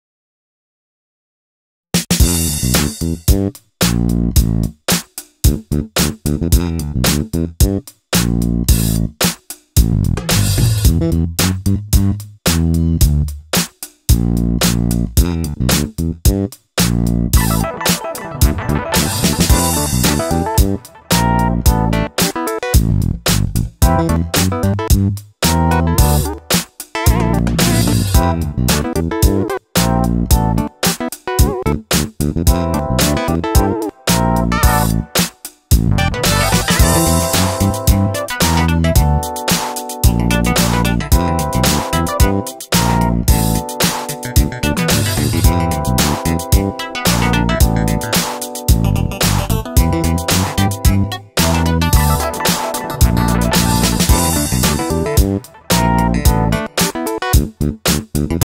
Lego 60191 Arctic Exploration Team
Summer 2018 set. New builds daily. Lego set 60191 Arctic Exploration Team Speed build.